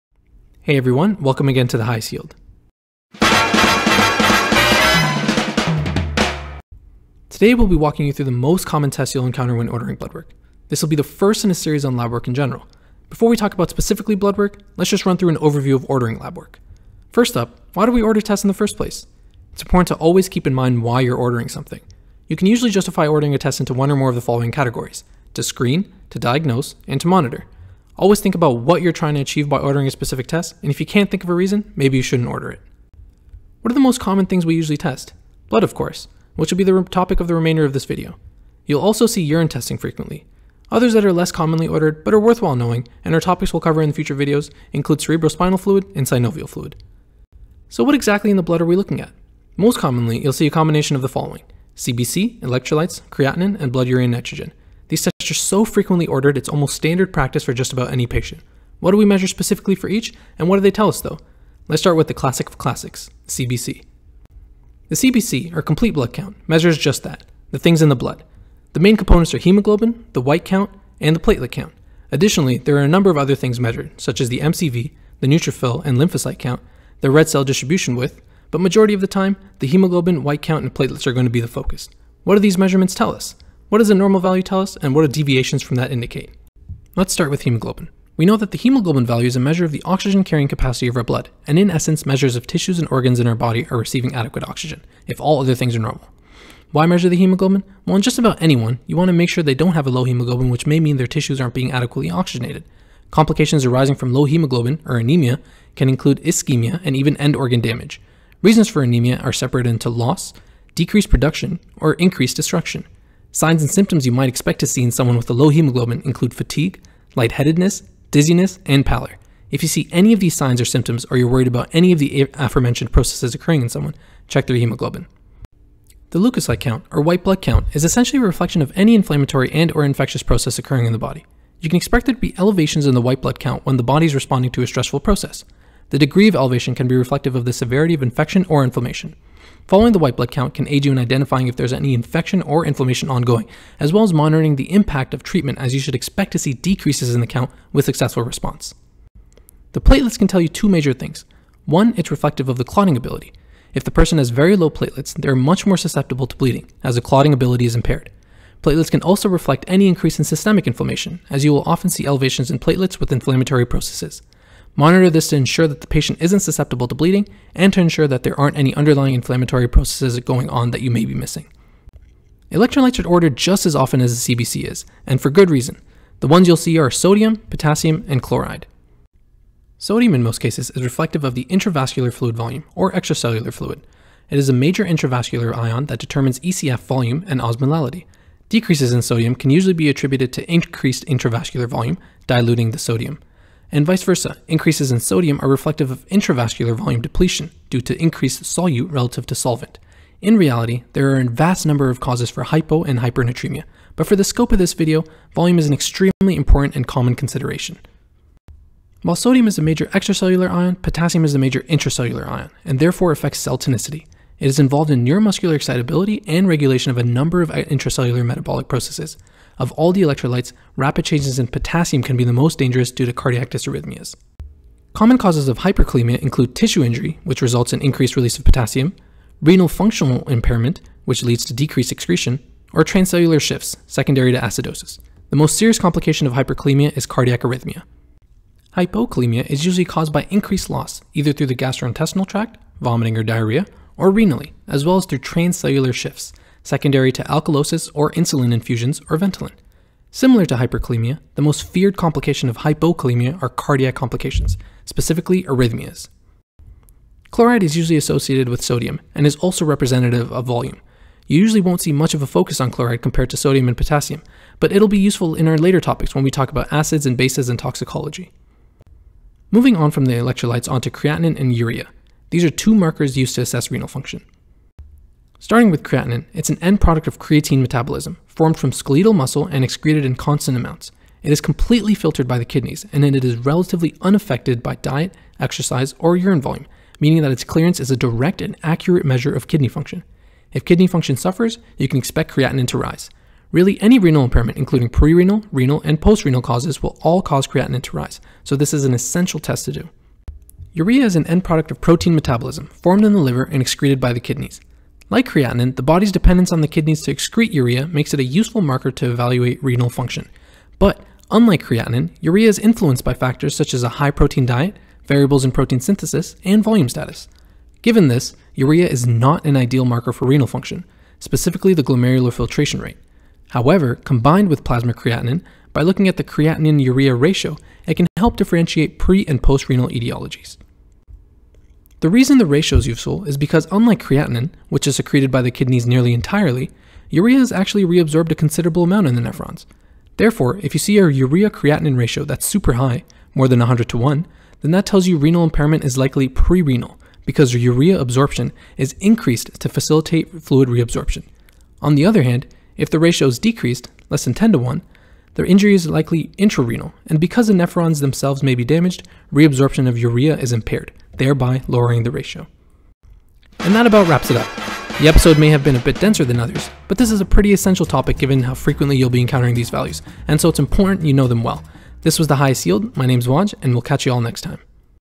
The Highest Yield - episode 3: CBC and electrolytes
Lab-work analysis guide by the lads at McMaster University